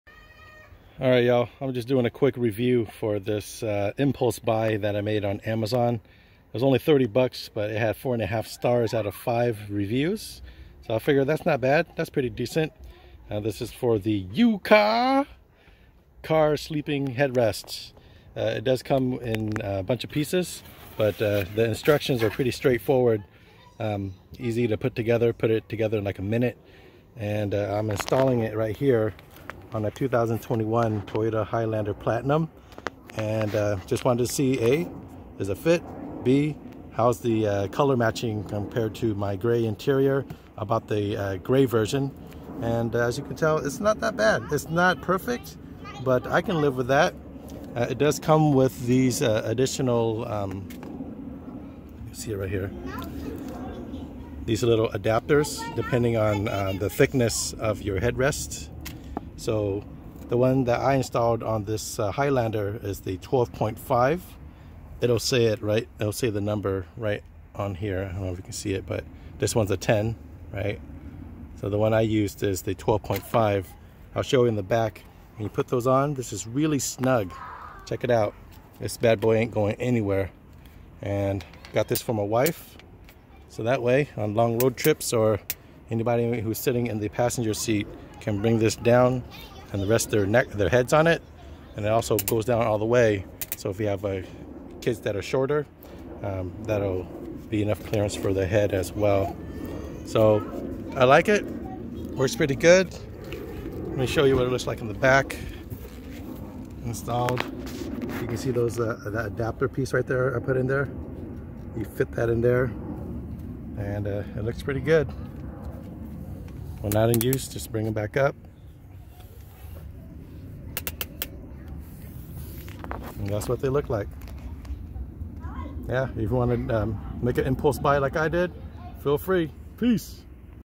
Yoocaa Adjustable Headrest/Neck support review installed on 2021 Toyota Highlander Platinum Hybrid
Impulse buy on Amazon! Fits snugly on the head rest after using the supplied 12.5 adapter pieces. Not a bad purchase for $30 with 4.5 star reviews. The headrest works great for younger kids when you adjust the headrest to the full down position.

LINK to product

Join our growing Facebook 4th Gen Toyota Highlander DIY/MOD community!

Thanh Tran